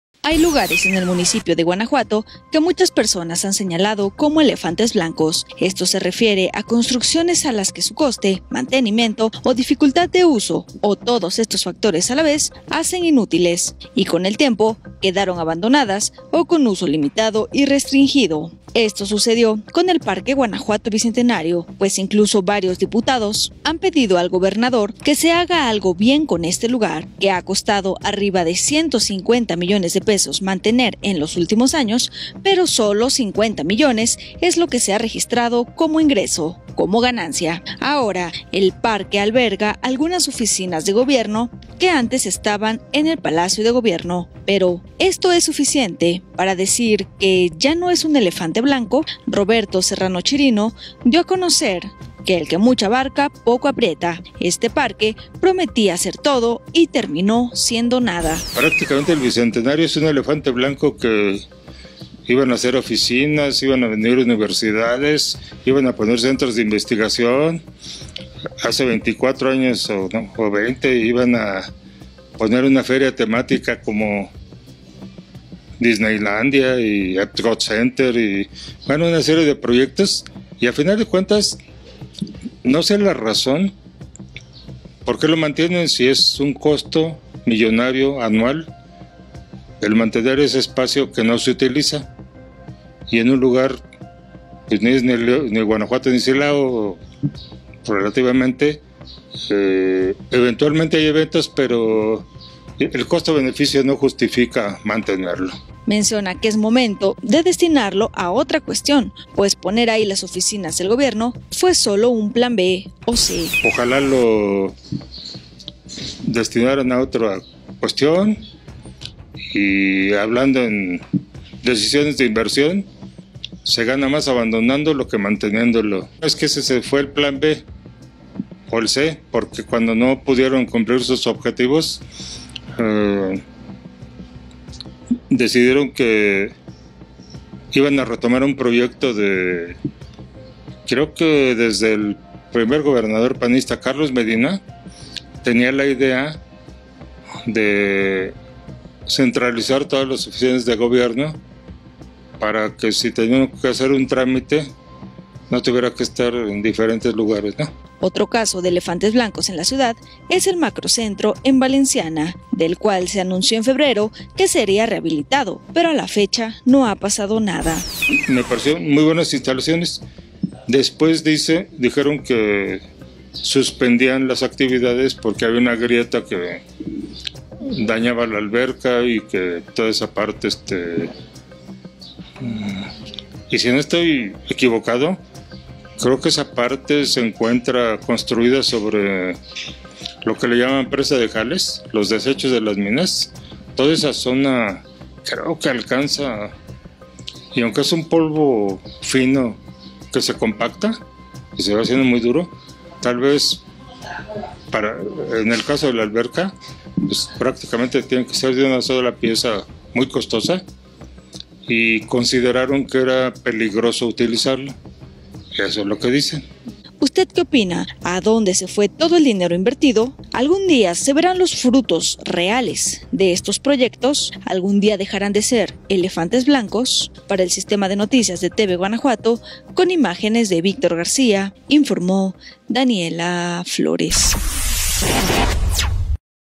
🐘🏢 ¿Qué pasa con los "elefantes blancos" de Guanajuato?
🐘🏢 ¿Qué pasa con los "elefantes blancos" de Guanajuato? Roberto Serrano lo explica.